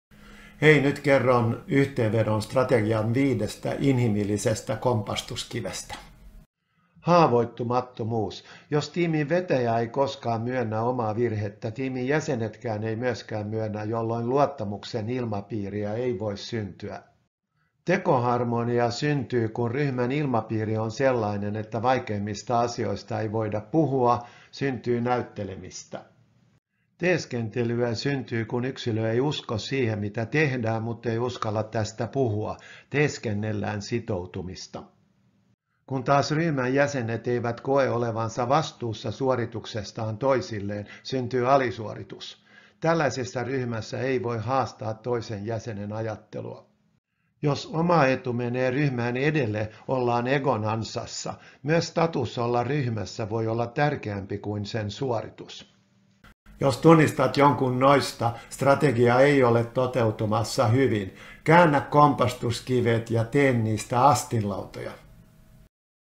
Viisi inhimillistä kompastuskiveä, Markus Westerlund
Tässä yhteenveto strategian viidestä inhimillistä kompastuskivestä.

Viisi kompastuskiveä

1. Haavoittumattomuus. Jos tiimin vetäjä ei koskaan myönnä omaa virhettä, tiimin jäsenetkään eivät myöskään myönnä, jolloin luottamuksen ilmapiiriä ei voi syntyä.
2. Tekoharmonia  syntyy, kun ryhmän ilmapiiri on sellainen, että vaikeimmista asioista ei voida puhua, syntyy näyttelemistä.
3. Teeskentely. Teeskentelyä syntyy, kun yksilö ei usko siihen mitä tehdään muttei uskalla tästä puhua. Teeskennellään sitoutumista.
4. Alisuoritus. Kun taas ryhmän jäsenet eivät koe olevansa vastuussa suorituksestaan toisilleen, syntyy alisuoritus. Tällaisessa ryhmässä ei voi haastaa toisen jäsenen ajattelua.
5. Ego ja status. Jos oma etu menee ryhmän edelle, ollaan egon ansassa. Myös status olla ryhmässä voi olla tärkeämpi kuin sen suoritus?

Jos tunnistat jonkun noista, strategia ei ole toteutumassa hyvin. Käännä kompastuskivet ja tee niistä astinlautoja.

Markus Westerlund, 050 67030